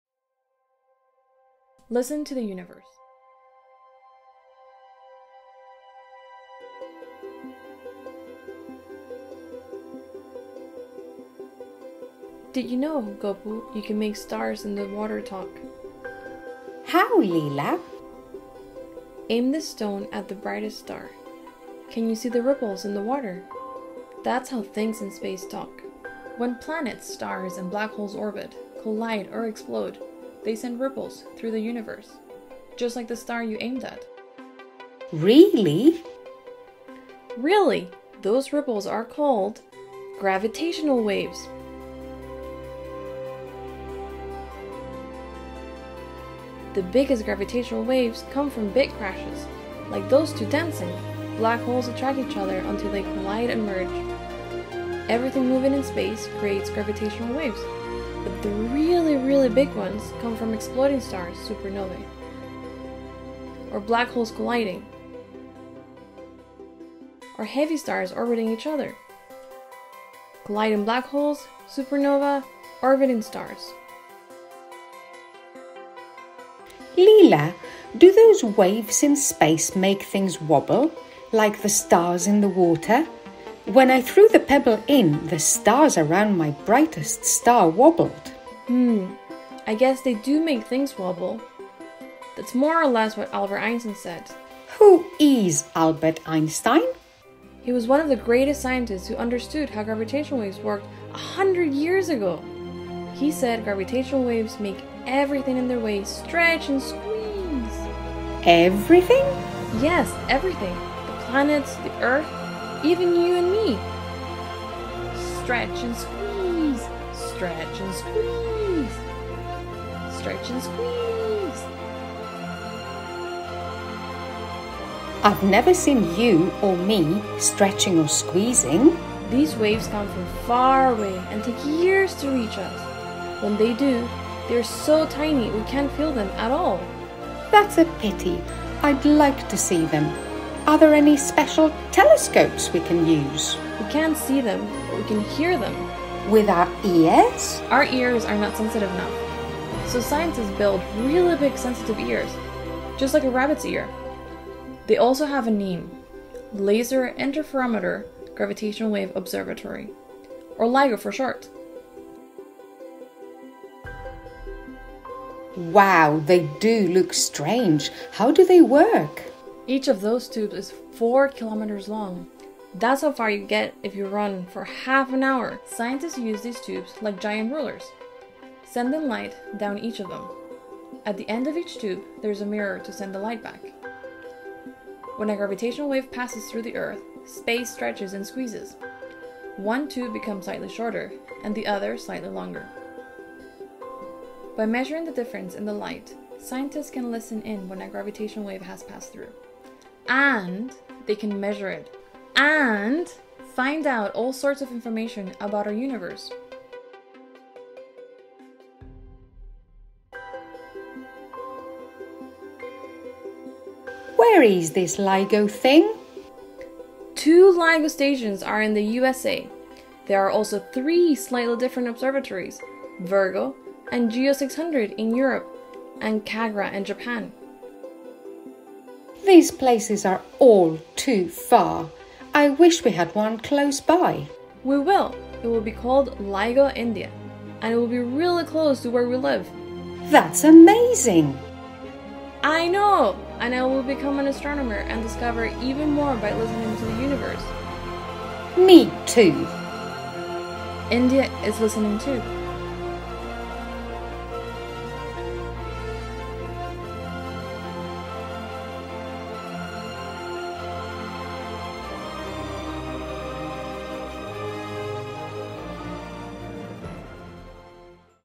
Pop-up book aims to help kids ‘Listen to the Universe’
An illustrated pop-up book which aims to teach children about the science of gravitational wave astronomy is setting out to make a splash in India.

‘Listen to the Universe’ is the result of a unique collaboration between Dr Mariela Masso Reid, an astrophysicist, and Dr Dimitra Fimi, a children’s literature expert from the University of Glasgow. They teamed up with an illustrator and scientists from LIGO-India to produce a children’s guide to the advanced technology which underpinned the historic first detection of gravitational waves in 2015.

A thousand copies of the book are being distributed to schools in the Hingoli district of Maharashtra state in western India, where LIGO-India, the latest of the world’s growing network of gravitational wave detectors, is currently in the early stages of construction.